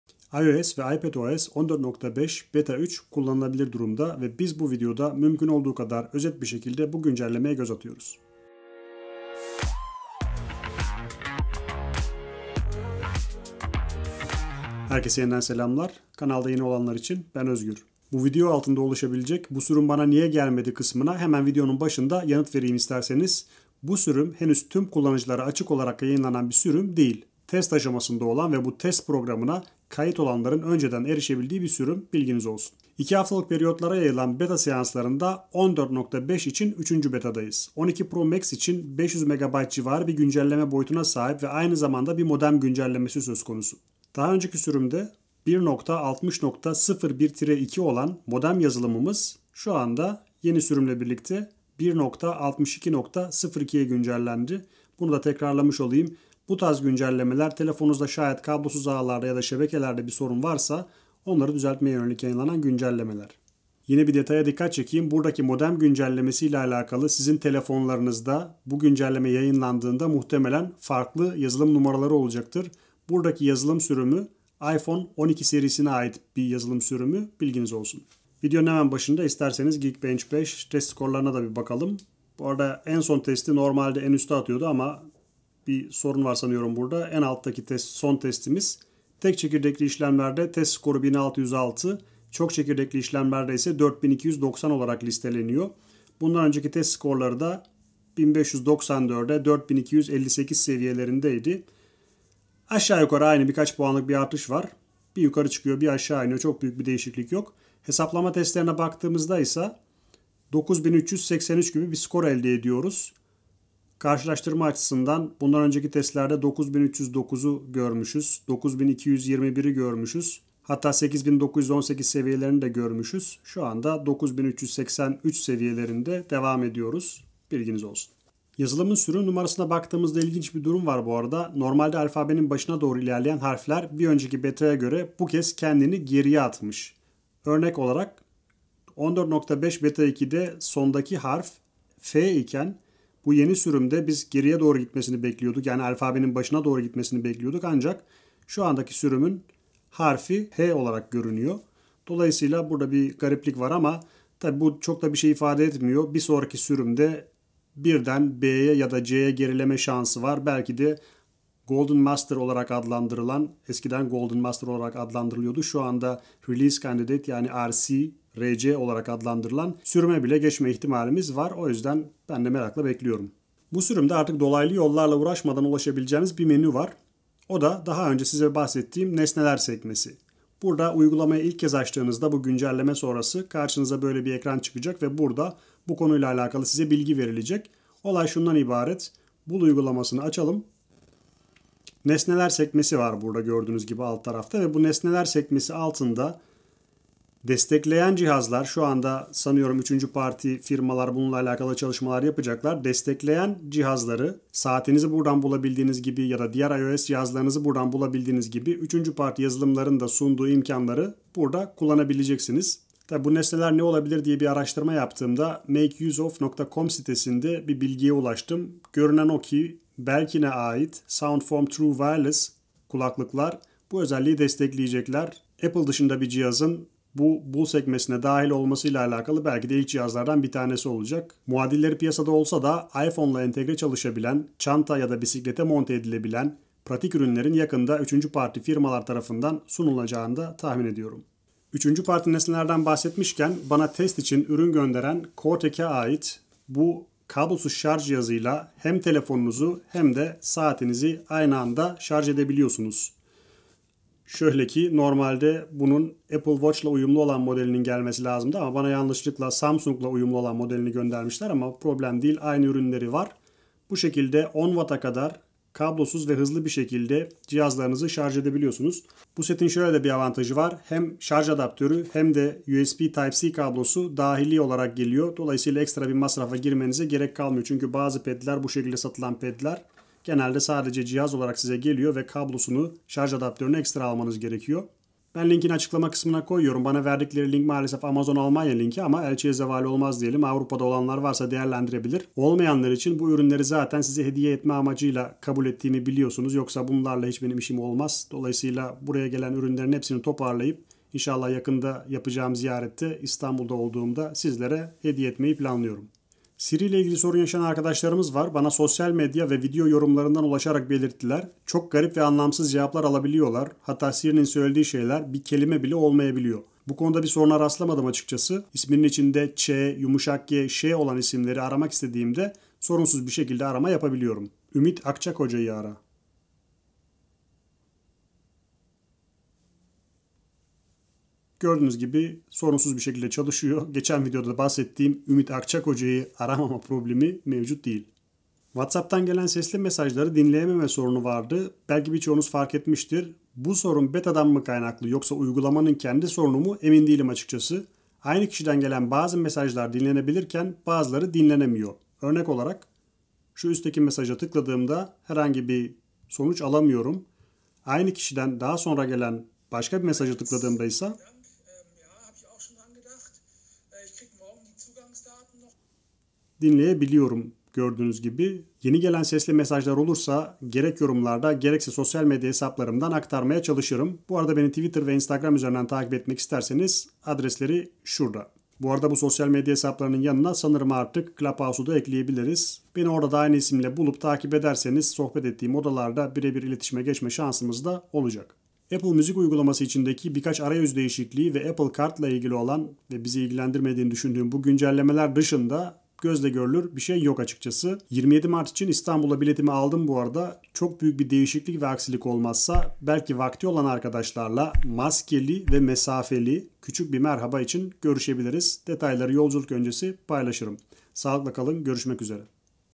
iOS 14.5 Beta 3 - Nesne Ekleme Özelliği
iOS 14.5 Beta 3, bul uygulaması üzerinde Apple ürünü hariç, 3. parti nesne eklemeye izin veren özelliği yayınladı. Bu videoda güncellemeye hızlıca göz atıyoruz.

Videoda bahsedilen Choetech ürün linki

Bu videoları izledin mi?